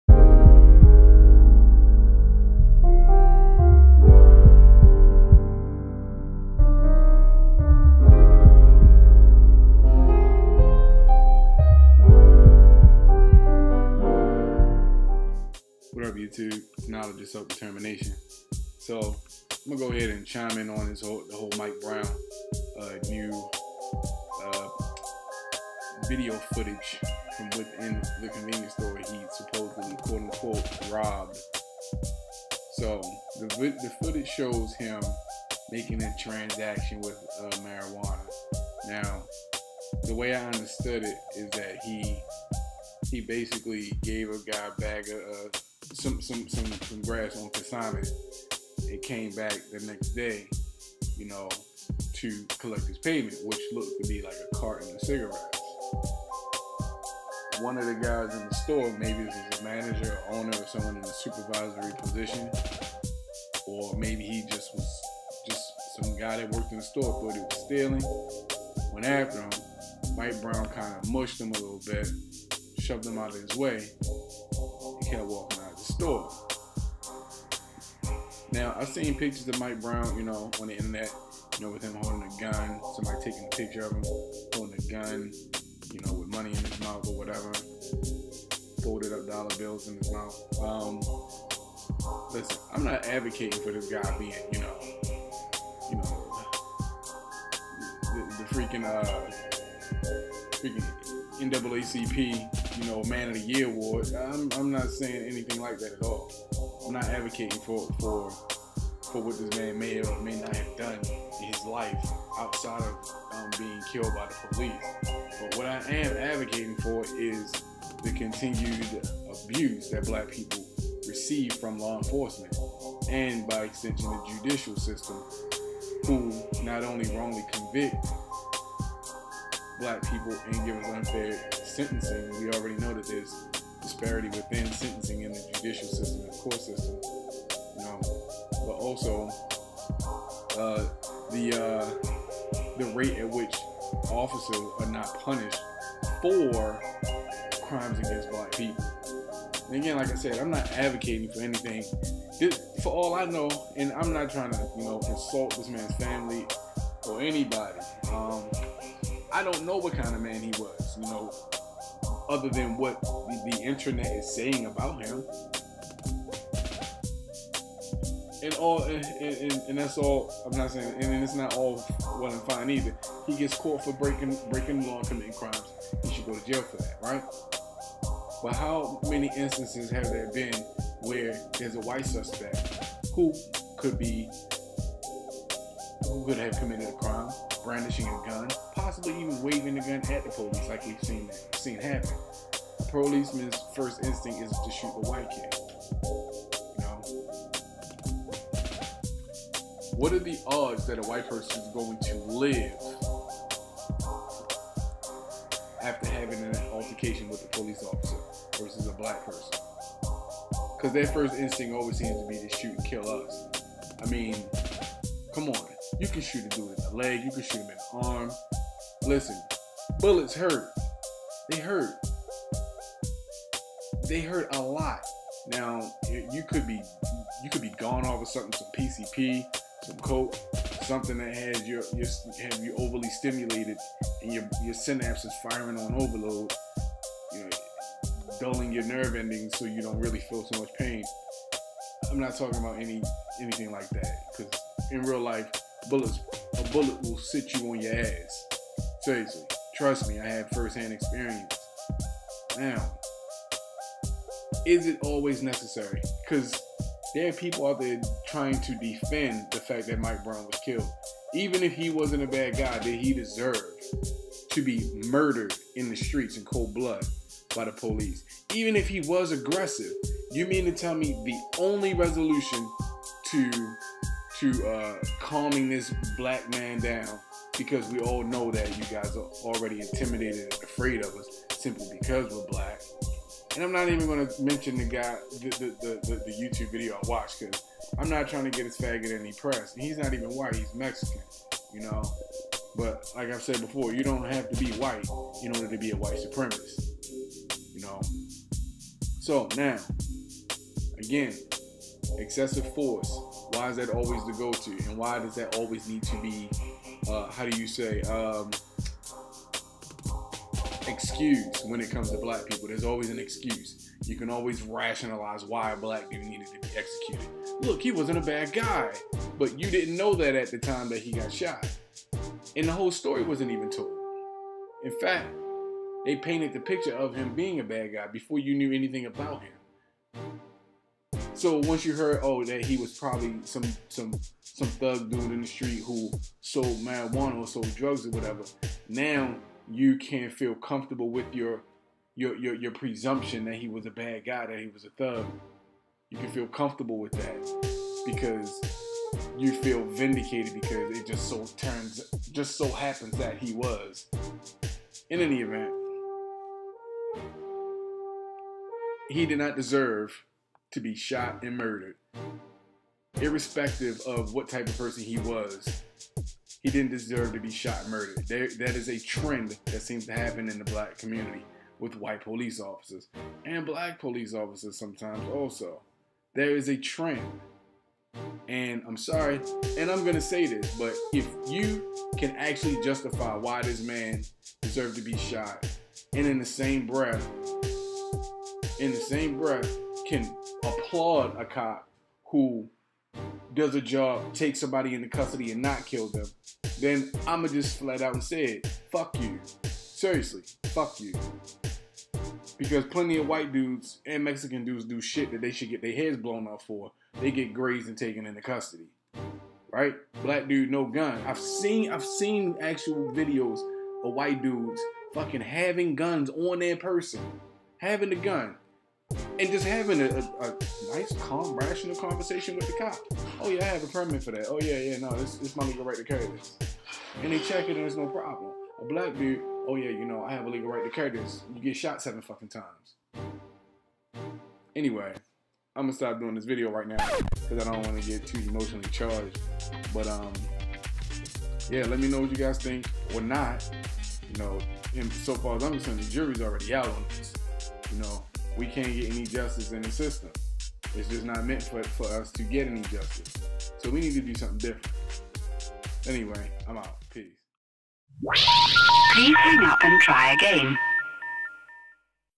Michael Brown, My Thoughts
It would seem that no matter what a white person does, no matter how fucked up of an individual they are, the police try as hard as they can to preserve the life of white people. However, when it comes to Black people, no matter what we do, we are all looked at as a threat that needs to be eliminated. No matter who they are, or their age.